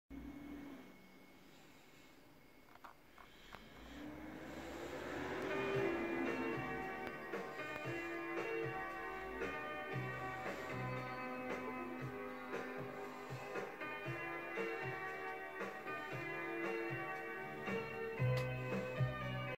A quick video to show our flake effect paints with a candy finish.